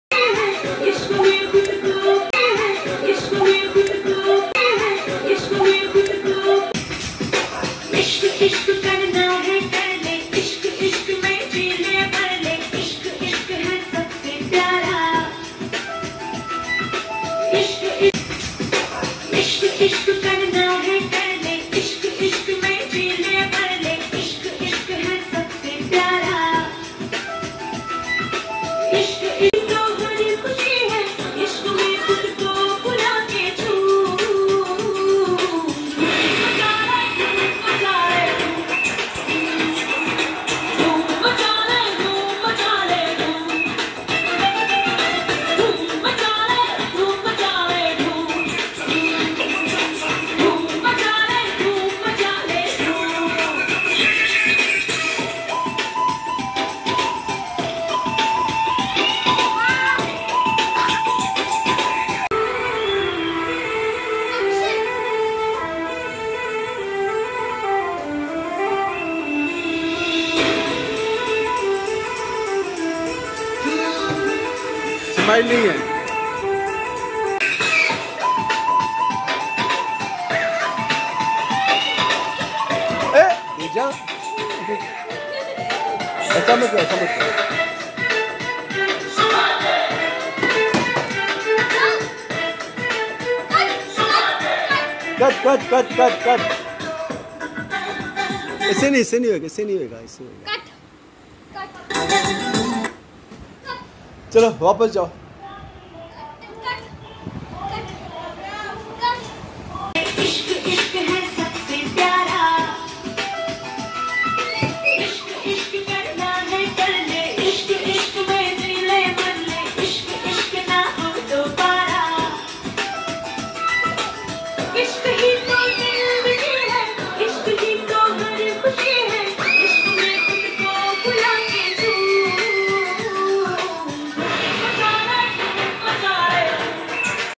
Stunts by two brothers age 7 & 2 year. Yugh & Atharv Kukreja Mumbai
Grandson of Suresh Kukreja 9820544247
Shooting at Home.
Bandra West Mumbai